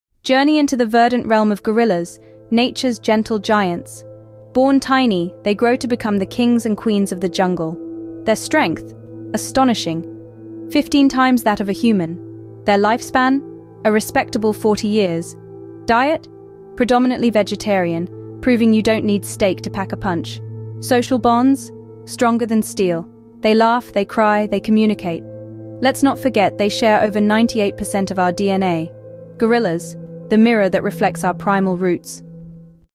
Gorilla Glimpse 40 Seconds of Wild
Welcome to our latest wildlife adventure! In this video, we dive deep into the intriguing lives of gorillas, the gentle giants of the jungle.